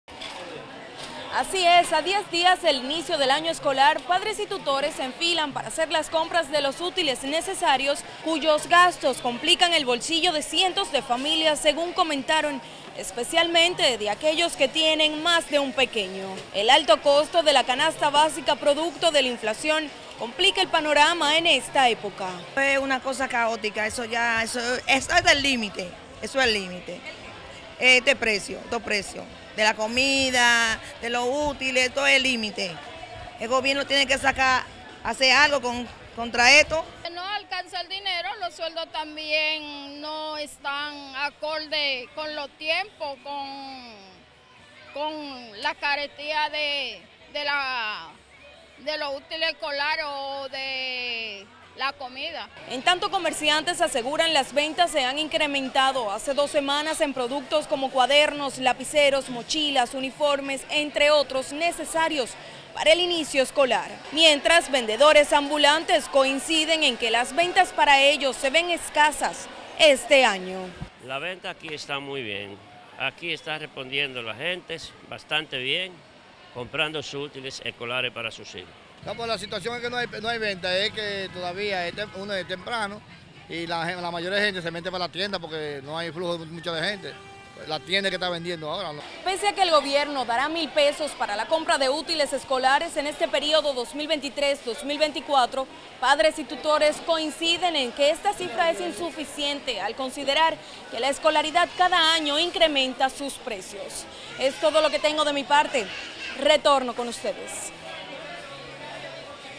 Se reactivan las compras de los útiles escolares
SANTO DOMINGO.- La compra de los útiles escolares comienza a reactivarse para el inicio del año escolar, mientras que los padres y tutores buscan alternativas ante el alto costos de los materiales.